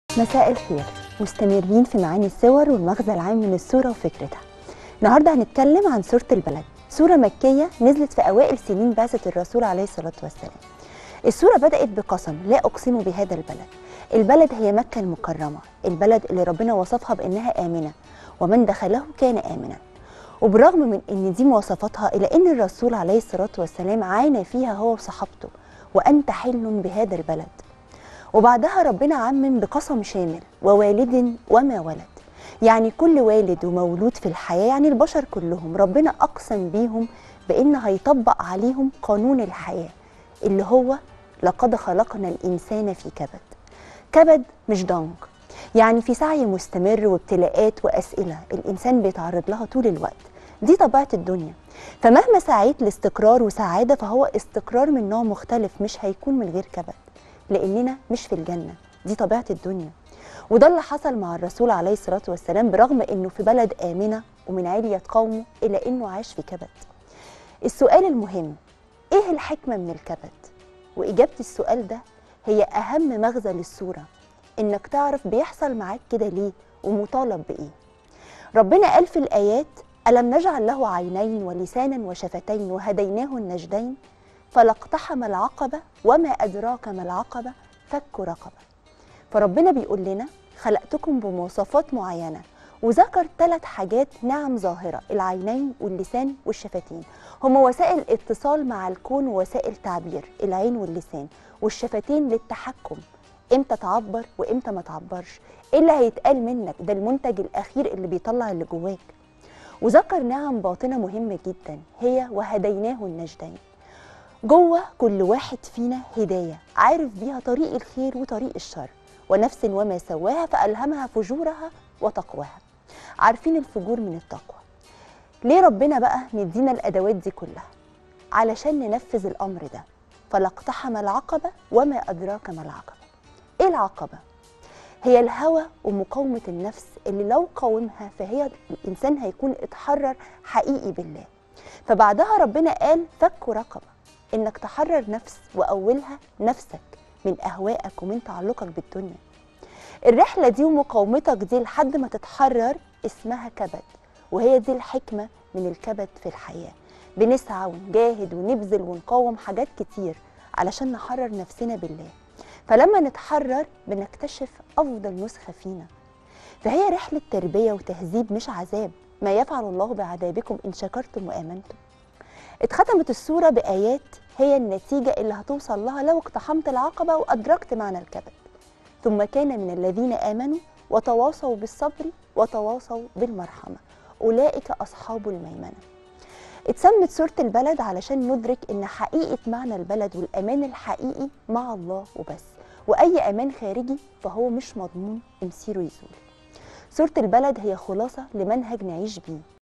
إيه هو الكَبَد وإيه الحكمة منه؟! وكيف يأخذ الشخص الحِكمة من سورة البلد؟!
إيه هو الكَبَد وإيه الحكمة منه؟! وكيف يأخذ الشخص الحِكمة من سورة البلد؟! سؤال النهاردة في حلقة جديدة من#وبه_نستعين